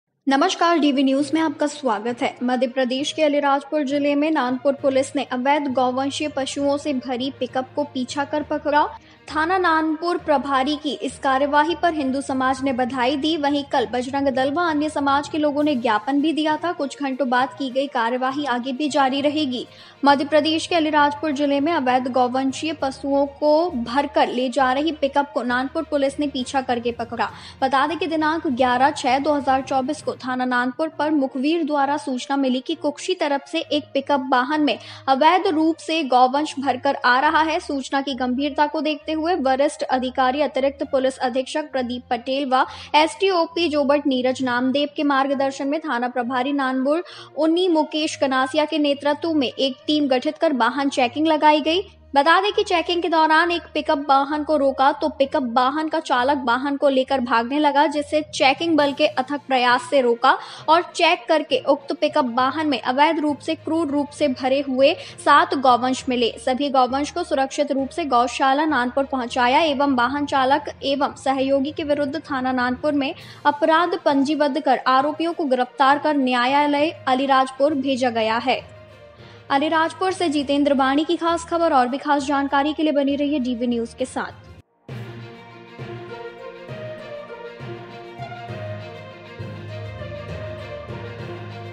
Alirajpur: Nanpur Police की बड़ी कार्यवाही, अवैध रूप से गोवंश से भरे वाहन को पकड़ने में पाई सफलता!
In Alirajpur district of Madhya Pradesh, Nanpur police chased and caught a crowd full of illegal bovine animals and dogs. Hindu society congratulated the Nanpur police station in-charge on this action, while yesterday Bajrang Dal and other community people resigned a few hours later. The action taken will continue in future as well. In Alirajpur district of Madhya Pradesh, Nanpur police chased and caught a pickup carrying illegal bovine animals. On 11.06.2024, Nanpur police station received information from an informer that an illegal vehicle carrying illegal bovine animals was coming from Kukshi. In view of the information report, a team was formed under the leadership of police station in-charge Nanpur Unni Mukesh Kanasiya under the guidance of senior officer Additional Superintendent of Police Mr. Pradeep Patel and S.D.O.P. Jobat Neeraj Namdev and vehicle checking was done. During checking, a vehicle was suddenly stopped and the driver of the pick up vehicle was going to drive away with the vehicle which was stopped by the easy efforts of the checking force and while checking, 07 cattle stuffed with illegal and cruel material were found in the pick up vehicle, all the cattle were safely taken to Gaushala Nanpur, a crime has been registered against the accused and the driver and his associate in Nanpur Police Station, the accused has been arrested and sent to the court of Alirapur.

About Channel:-

Channel Links:-